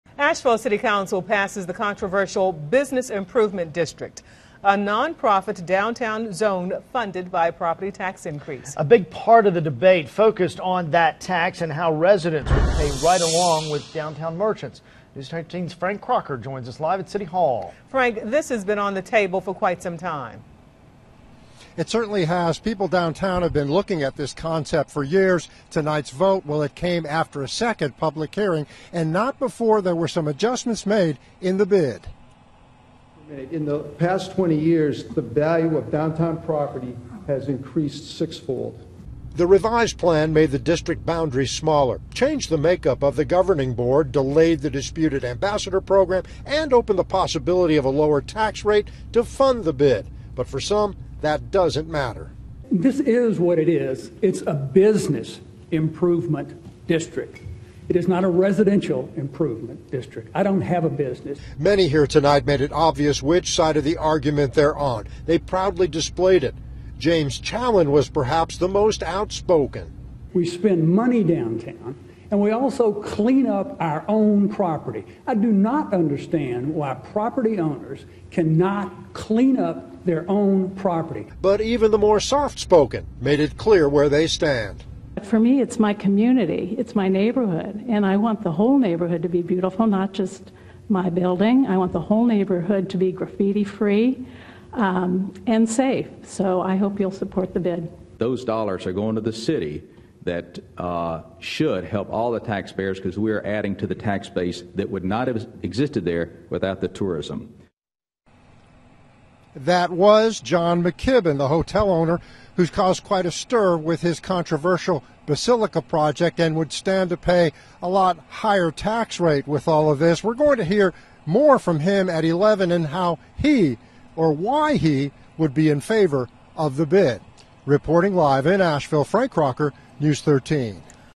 Asheville City Council Passes Controversial BID Proposal
Asheville City Council passes the controversial Business Improvement District proposal plan, a non-profit downtown zone funded by a property tax increase. A big part of the debate focused on that tax and how residents would pay right along with downtown merchants. People downtown have been looking at this for years. Tuesday's vote came after a second public hearing and changes in the proposal. The revised plan made the district boundaries smaller, changed the make-up of the governing board, delayed the disputed "ambassador" program and opened the possibility of a lower a tax-rate to fund the "BID". \nFollow Frank Kracher on Twitter @FrankKracher